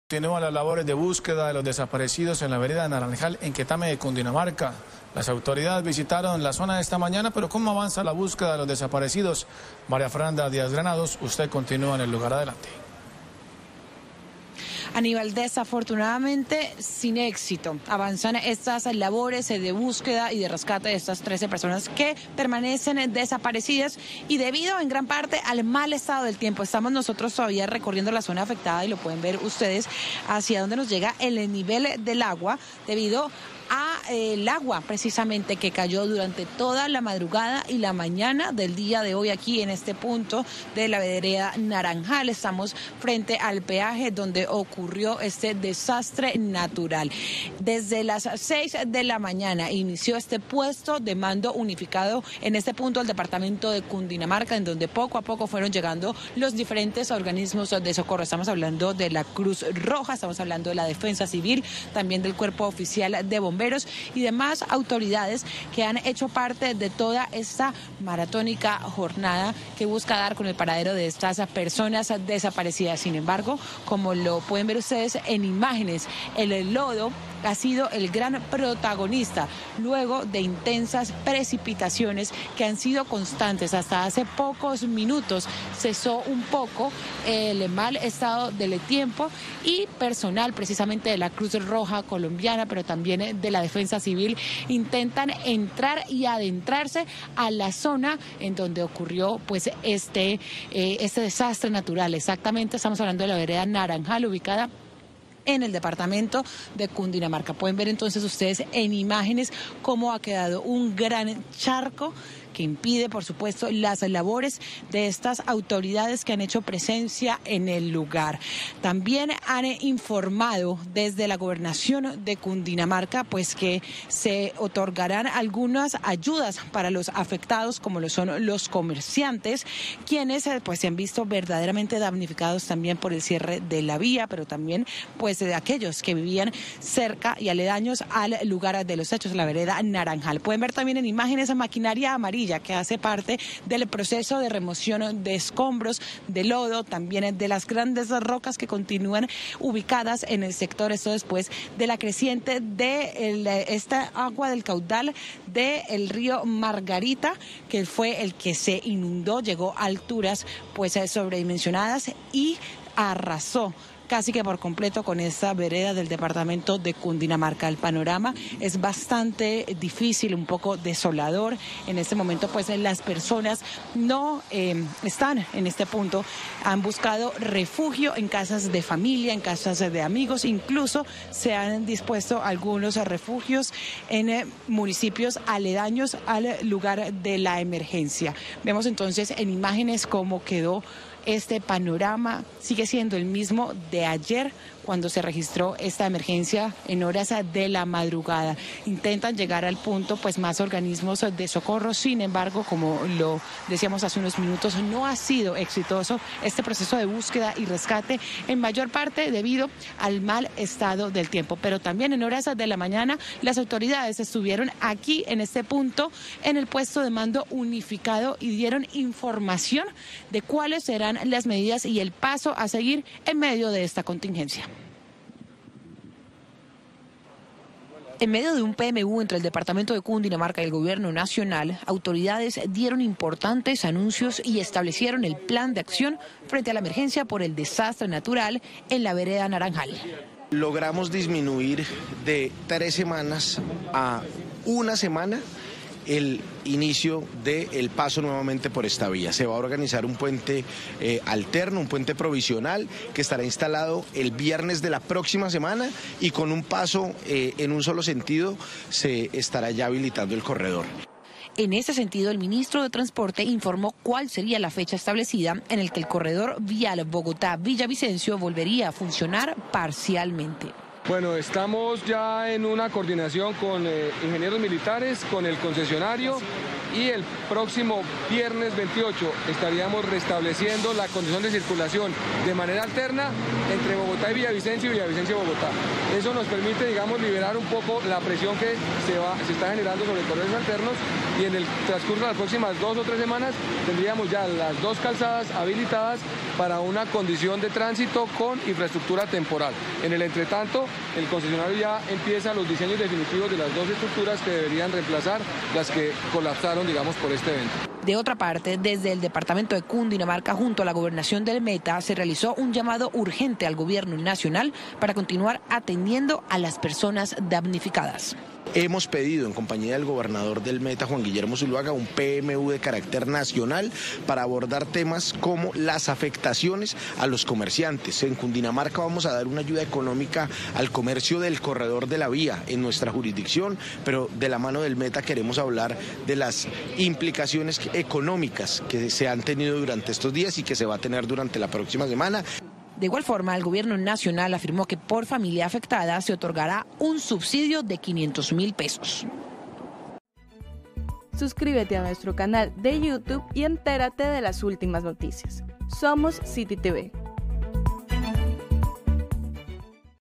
Autoridades toman medidas para atender emergencia en Quetame | CityTv
Autoridades continúan con las labores de búsqueda y rescate de las personas que permanecen desaparecidas tras la avalancha presentada, el pasado martes 18 de julio, en la vereda Naranjal, jurisdicción de Quetame, Cundinamarca. Según el reporte entregado, hasta el momento se han hallado 15 cuerpos sin vida. Conozca más detalles en la nota informativa.